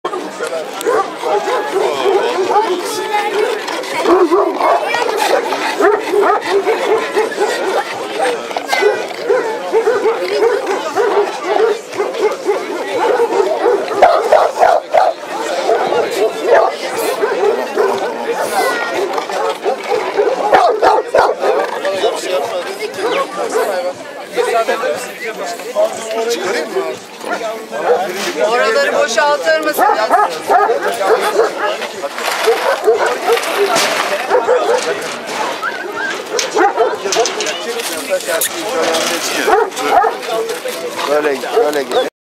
Dogue de bordeaux
Bordo bulldoğu fransanın bordeaux şehrinle meşhur köpeği bana göre bu kötü bir örnek,çok daha iyileirni gördüm